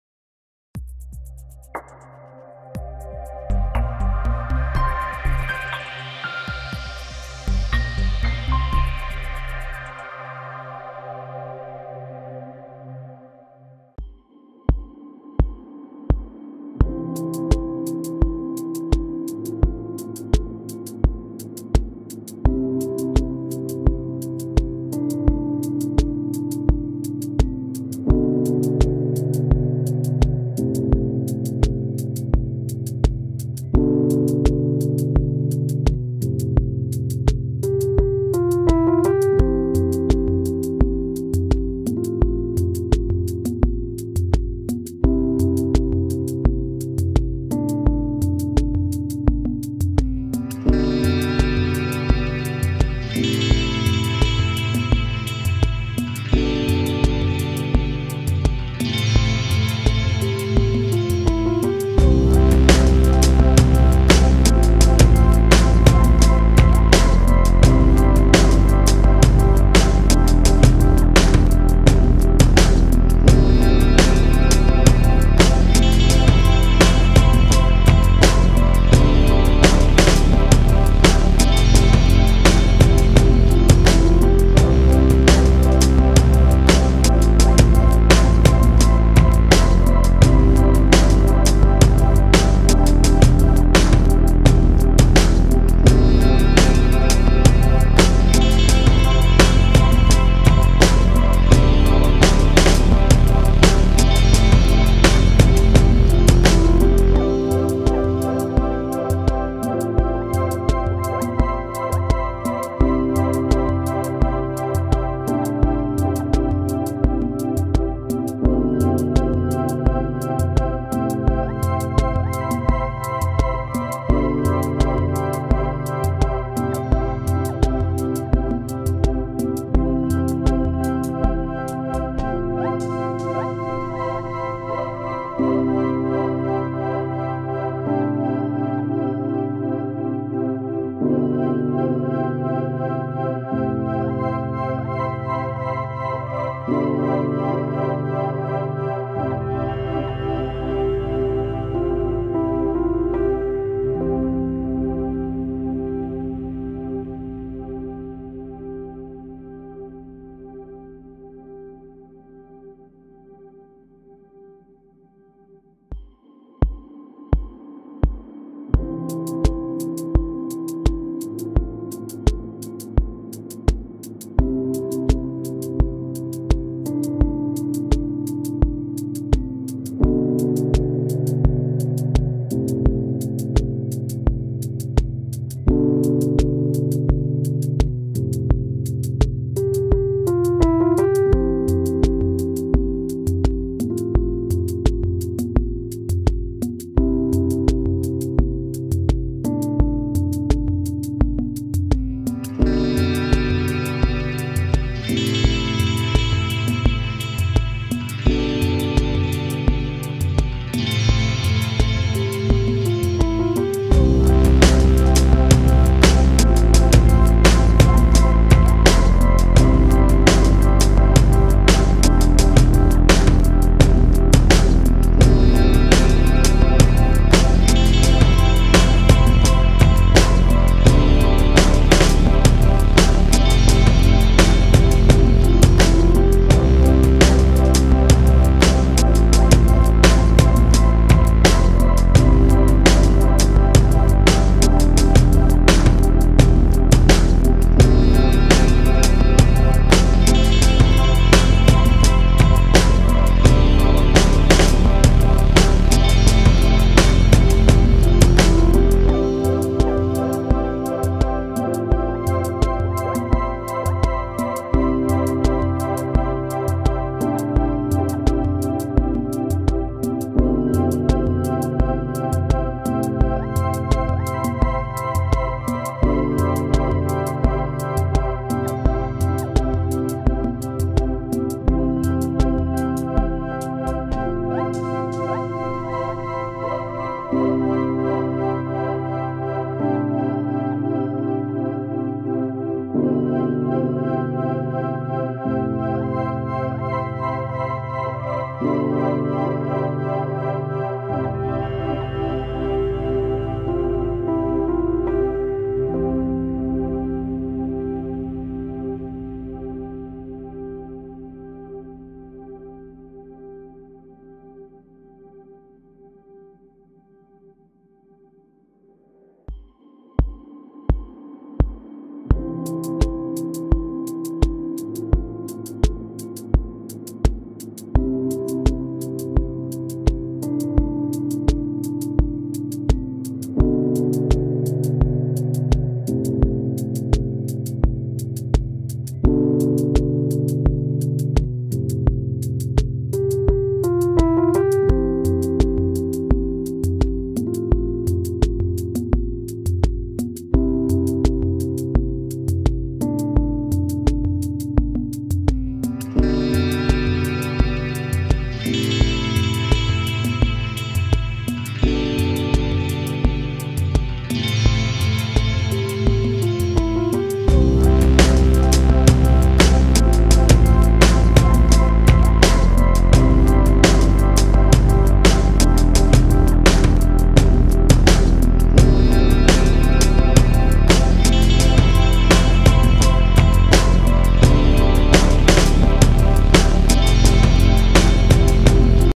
how to make a planet in photoshop
Design a Planet for your business it helps your photoshop tool make easy
for beginners and learn photoshop use different tools in this video

I hope you will learn the easy way to use photoshop
all links are here which use in this tutorial